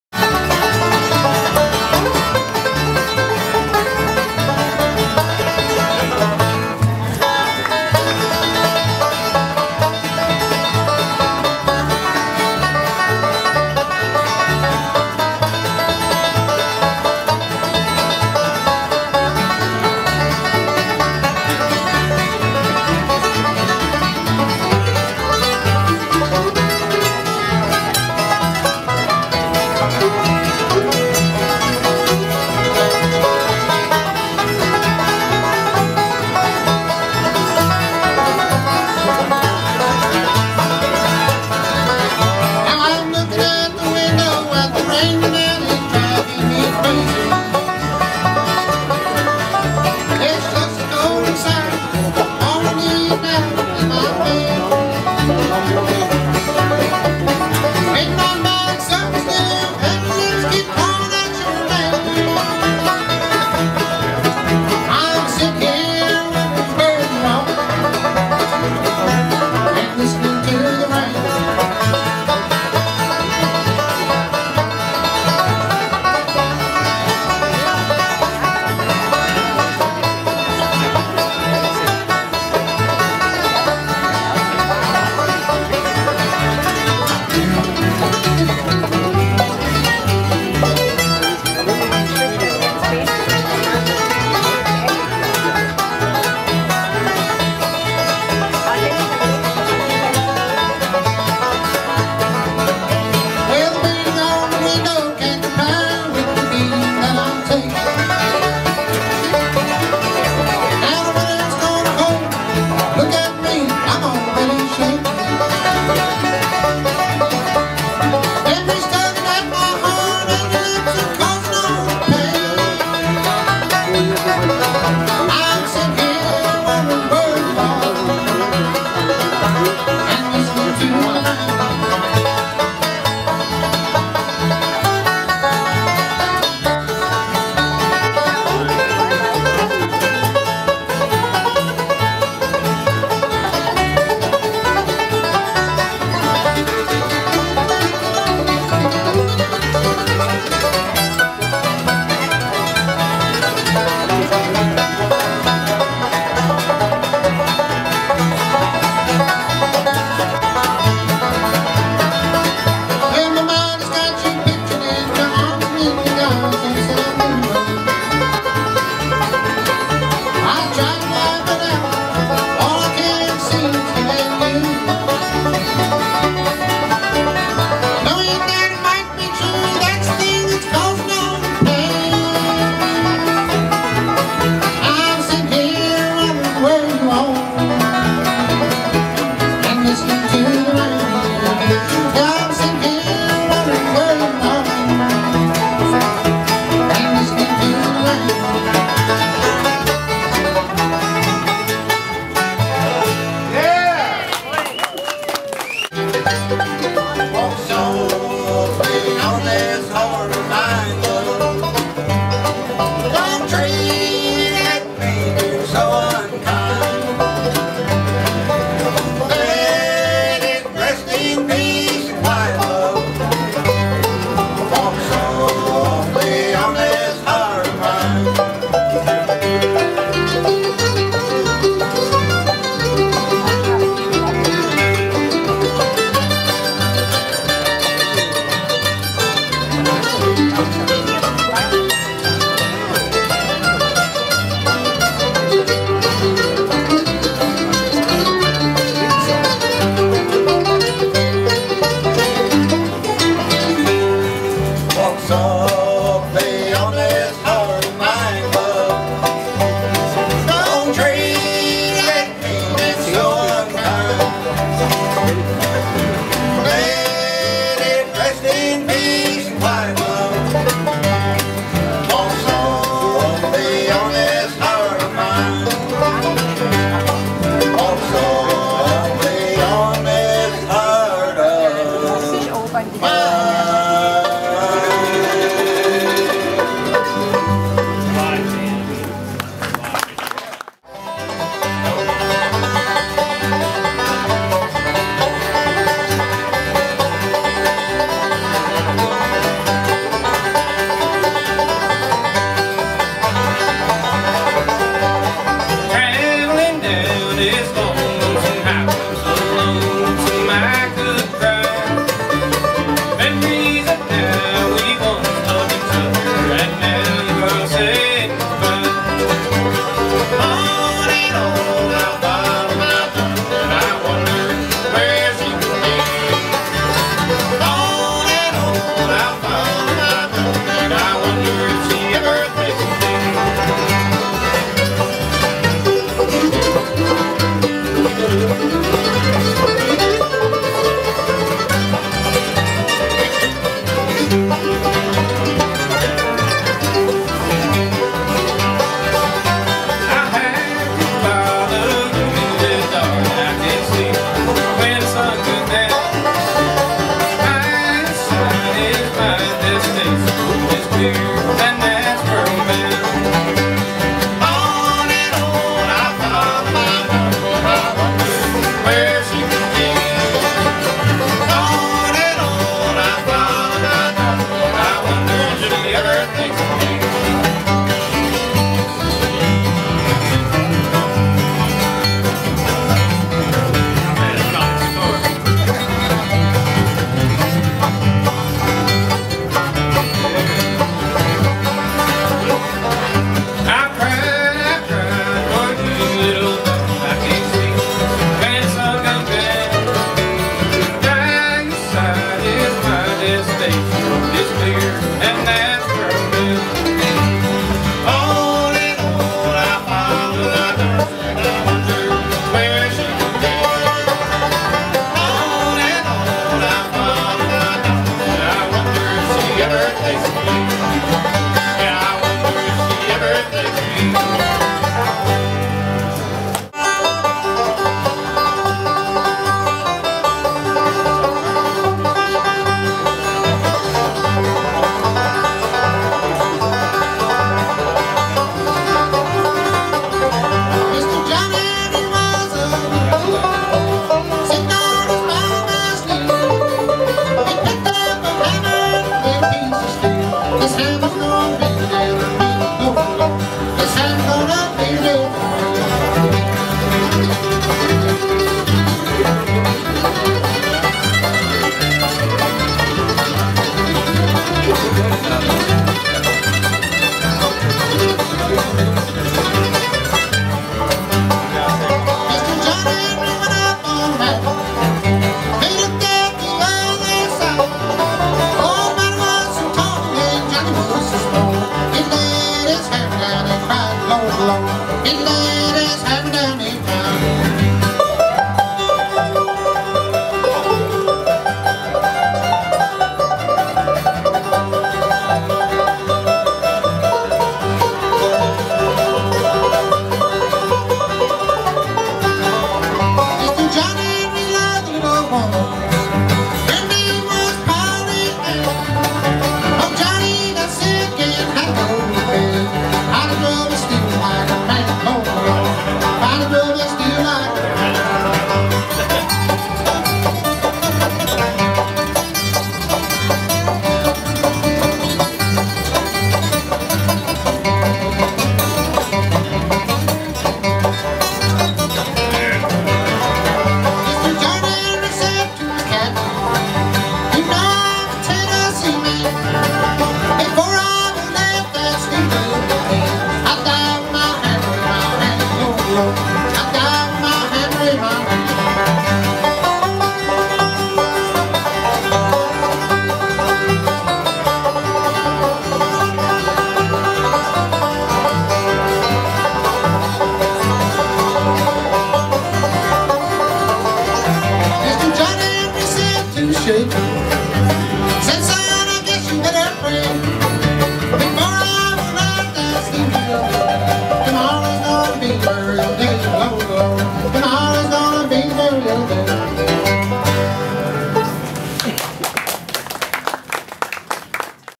Belaire Boys Bluegrass Sept 2010
A few songs from the Belaire Boys Concert in Millville, NJ on September 12, 2010